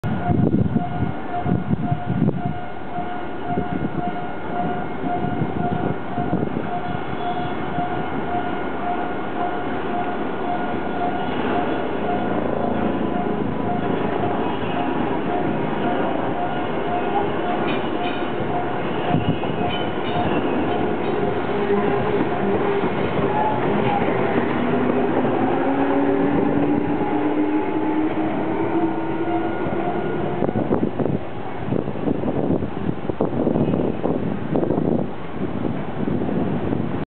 静岡鉄道静岡清水線1000系電車 新静岡～日吉町
新静岡駅を発車して日吉町駅に向かう新清水行き普通列車